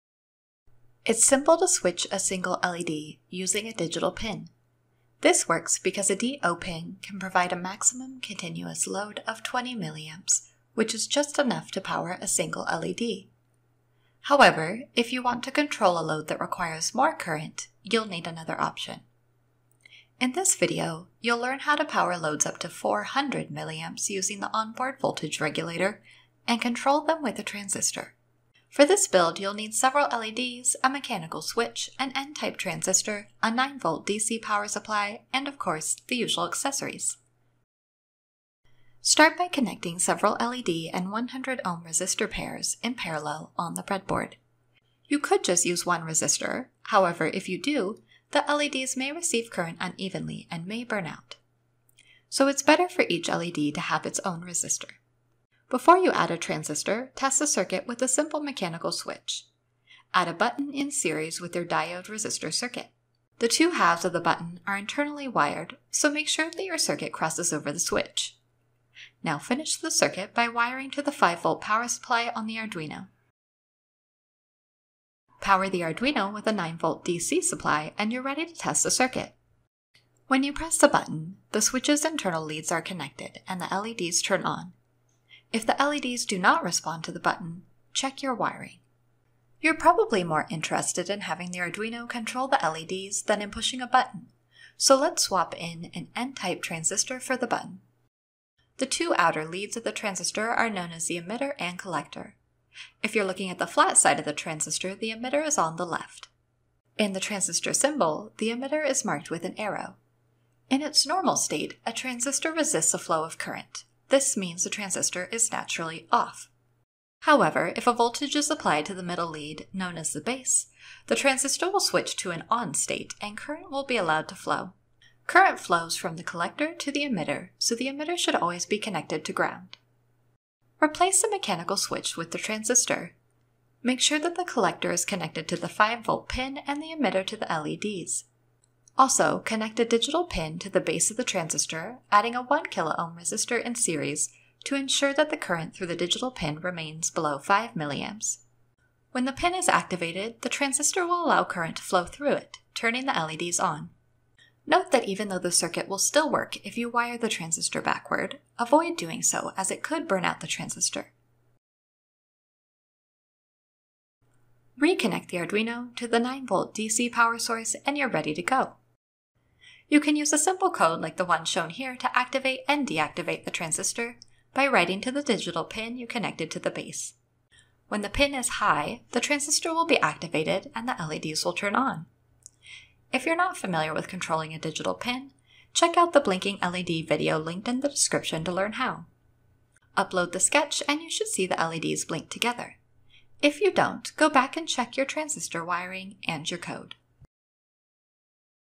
How to use a transistor as a switch
In this video, you'll learn the basics of how to use an Arduino to power and control loads using buttons and transistors.
If you're not familiar with using a digital output pin on the Arduino, get started by watching the first video in this series, linked below.